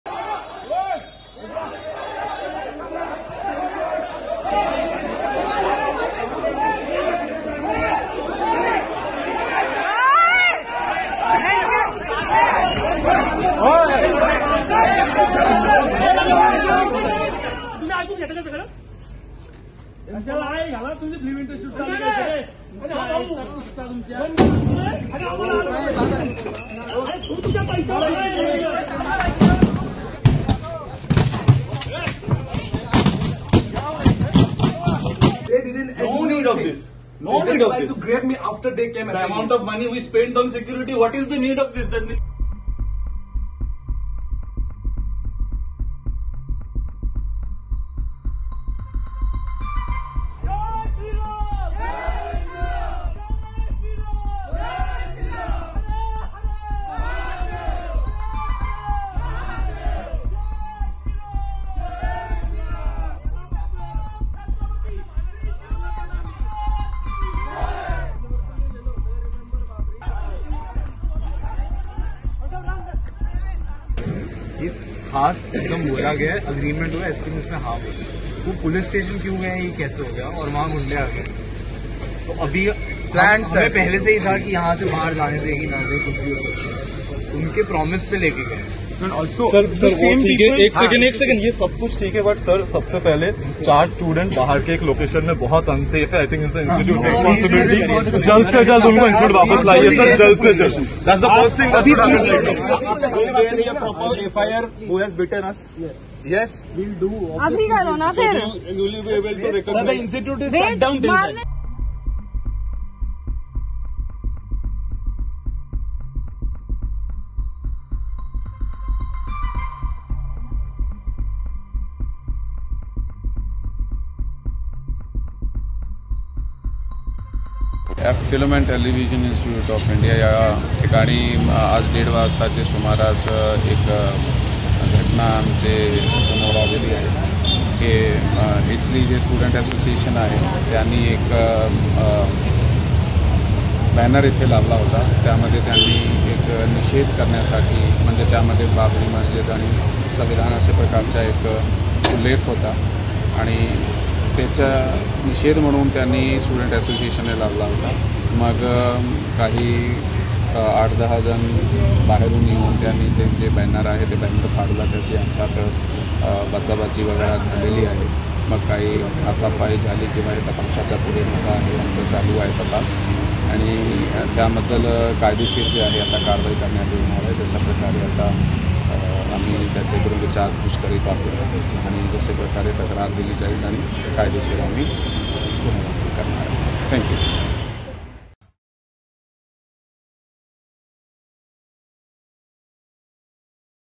Clash Erupts At FTII Pune Over Controversial Banner; Right-Wing Hindutva Groups Set It Ablaze
FTII Pune News Today: Tension flared at the Film and Television Institute of India (FTII) in Pune as a clash broke out between members of Hindutva outfits and FTII students over a banner denouncing the Babri Masjid demolition as against the Constitution. The police reported that right-wing Hindutva members set the banner on fire, leading to a heavy police deployment inside and outside the campus. Riot control vans and quick response teams were mobilized to address the situation. The incident occurred around 1.30 pm, following a film screening and exhibition related to the Babri Masjid-Ram Janmabhoomi issue organized by the FTII Students' Association. An FIR was registered against 12 to 15 individuals associated with multiple Hindutva right-wing groups based in Pune, as investigations to identify them are underway.